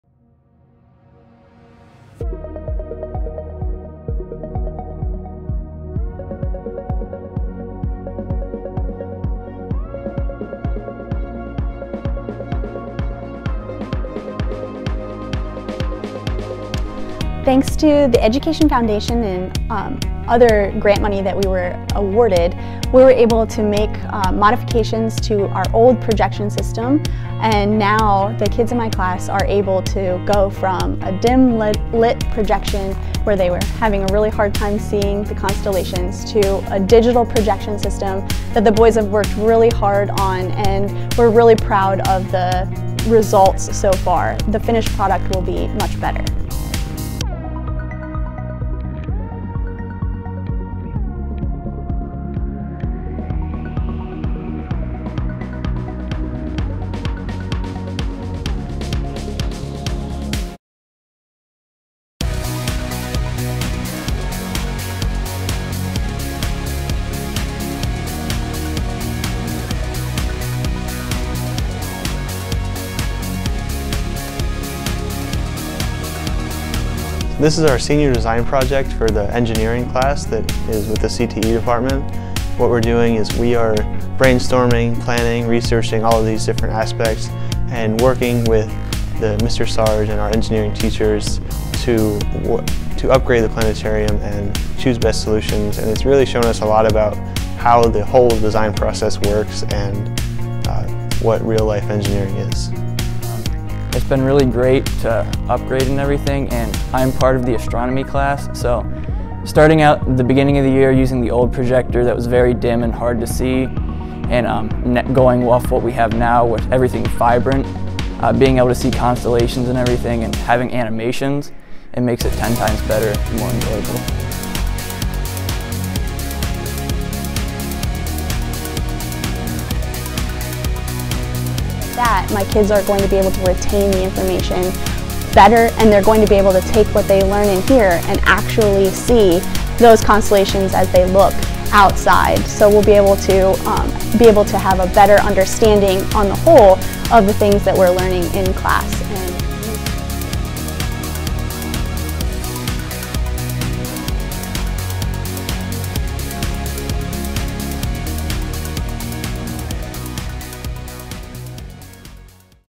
Out of this World: Planetarium Project in Motion
The high school’s planetarium is nearing its 50th year in operation. While functional, it has seen little if any upgrades since the building opened in 1972 and faces the risk of irreparable failure. A short-term solution to upgrade the physical aspect of the astronomy curriculum is in the works in conjunction with the science department and WAHS seniors LJ Boone and Cameron Crossley. Boone has engineered a plan that would use a curved mirror, large projector and a fish-eye lens mounted to the current planetarium dome to give the department the capability to run a digital program on the dome. While a short-term upgrade, it’s been verified that aspects of this project could be embedded in any future, more comprehensive upgrades.